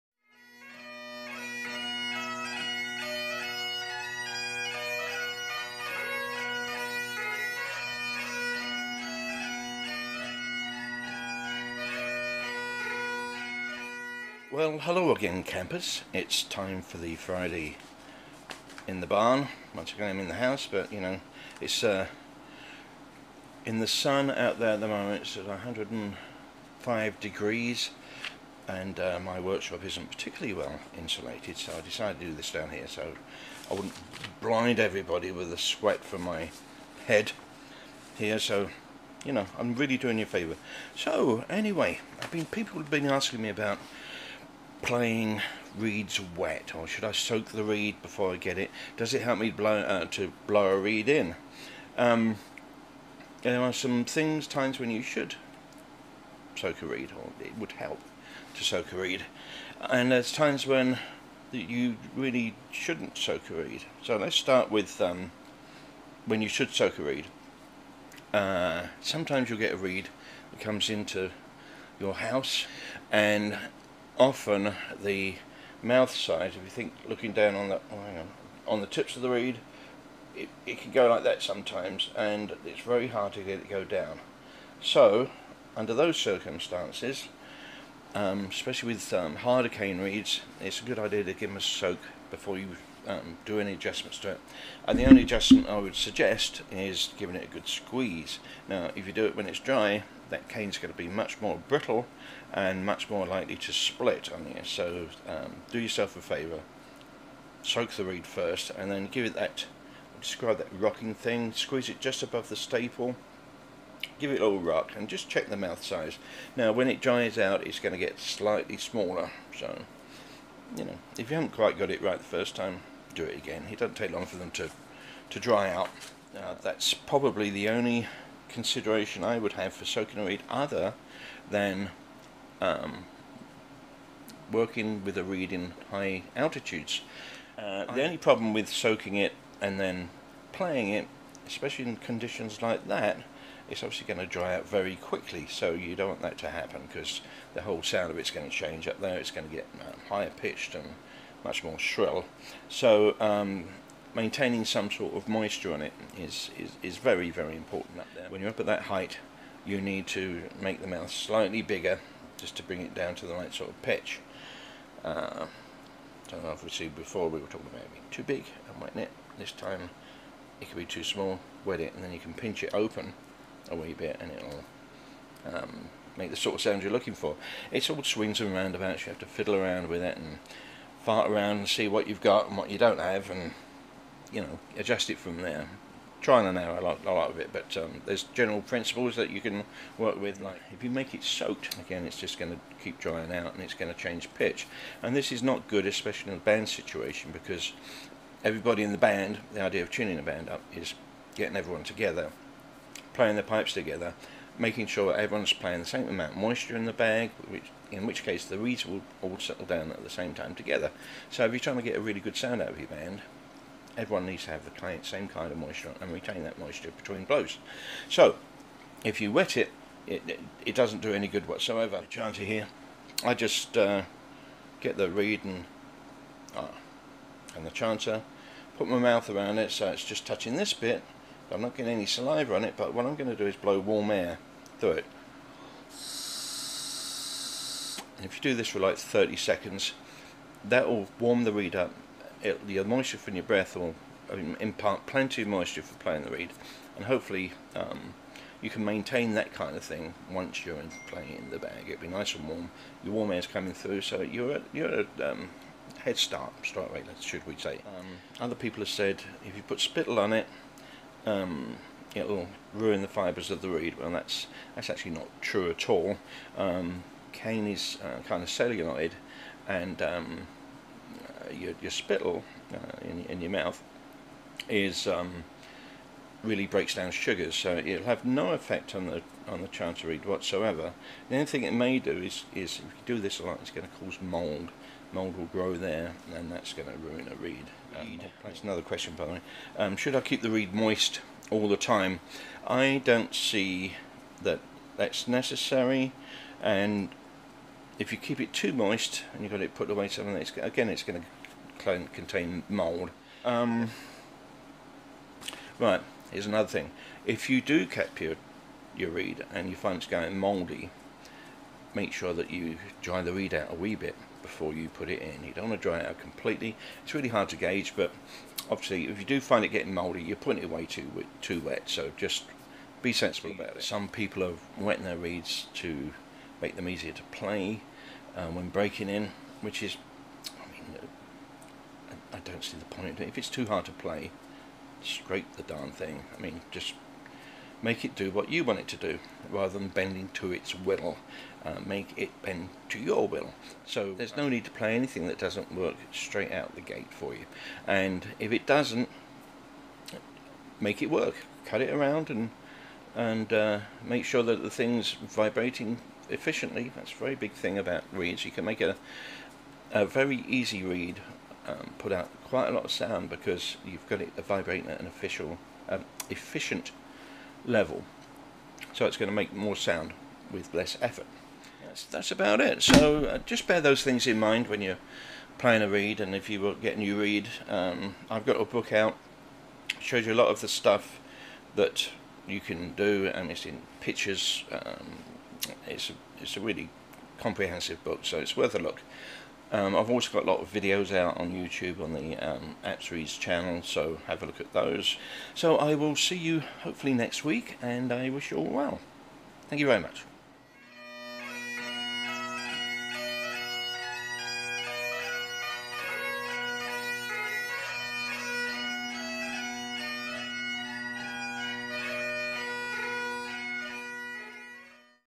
Bagpipes. In The Barn. Episode 31 - Should I soak my reed?
Chris answers viewers questions about when you should soak your chanter reed.